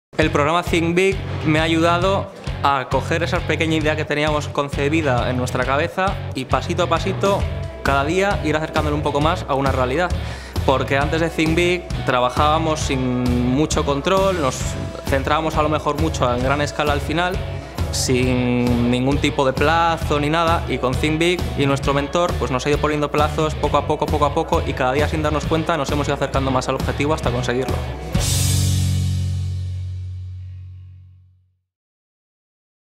Think Big: El paso de Álvaro San Juan por el programa en 20 segundos
Álvaro San Juan define su paso por el programa Think Big como una experiencia que le ha permitido que una idea se haga realidad gracias a la metodología y al apoyo de su mentor.